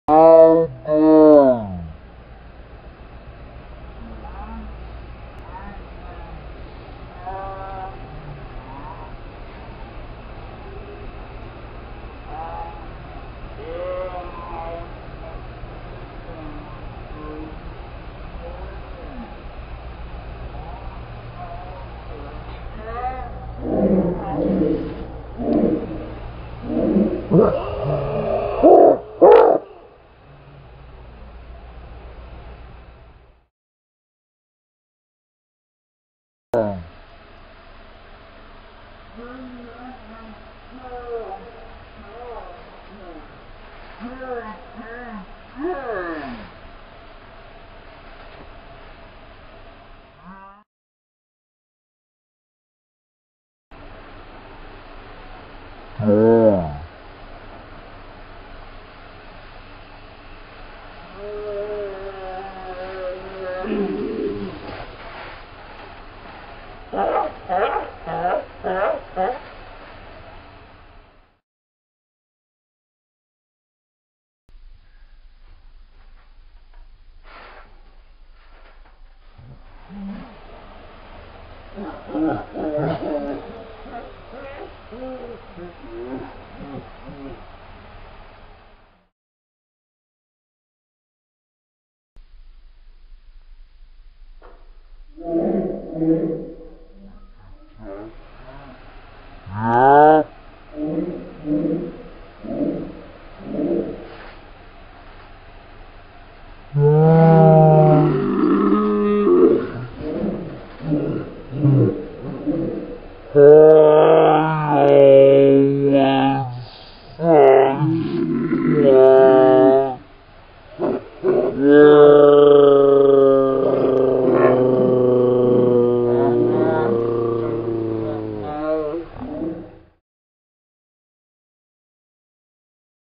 Slow-mo tackling pillows on tramp
I know we are going to have more better and fun videos on there way # if you want a slow mo app just get flow you can choose what ever mode of speed you want to go on. I hope  you enjoy the video peace.✌️